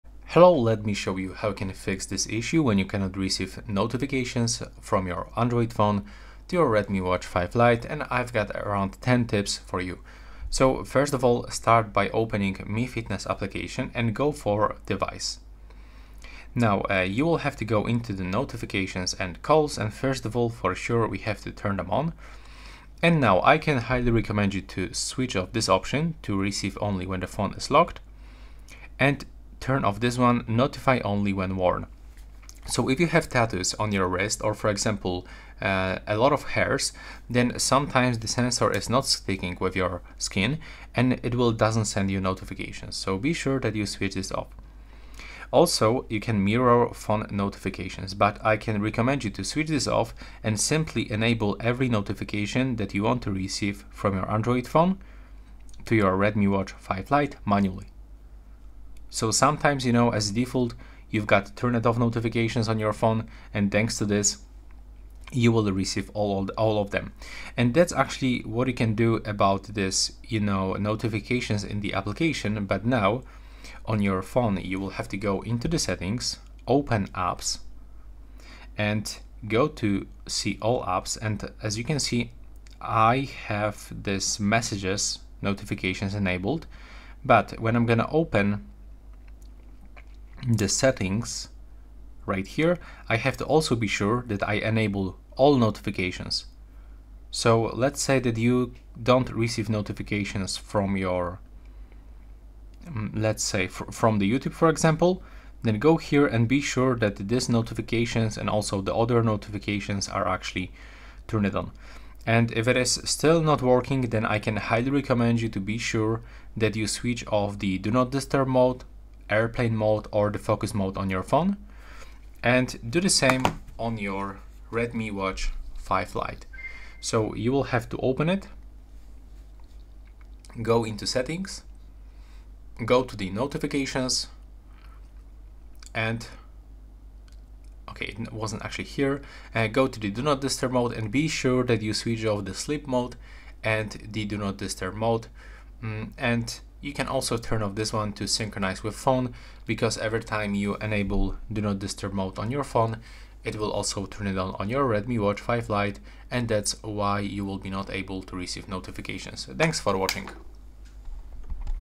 Redmi Watch 5 Lite: How to Fix Notifications Problems
In this video, we’ll explain how to fix notification problems on your Redmi Watch 5 Lite. If you’re not receiving alerts or updates, we’ll guide you through troubleshooting steps to restore proper notifications. Learn how to resolve common notification issues on your Redmi Watch 5 Lite for a smoother experience.

I'll do my best to reply promptly and assist you.